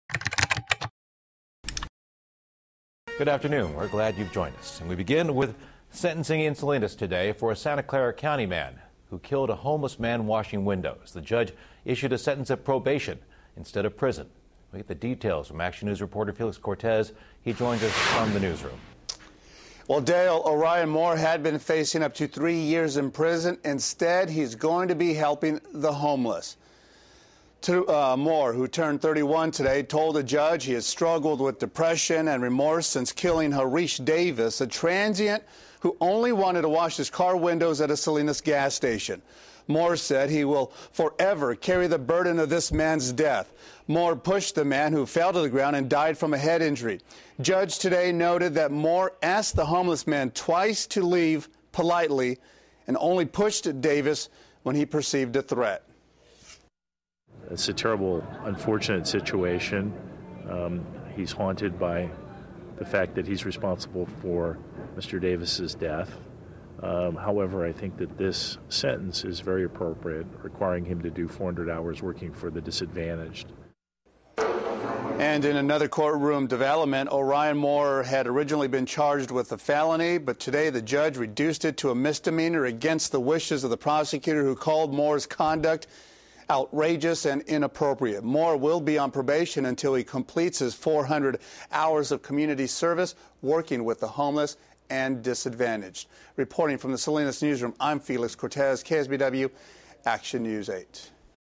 Man Sentenced In Transient Death At Salinas Truck Stop
A man who pleaded no contest in the death of a homeless man was sentenced to 400 hours of community service, which includes working with the homeless.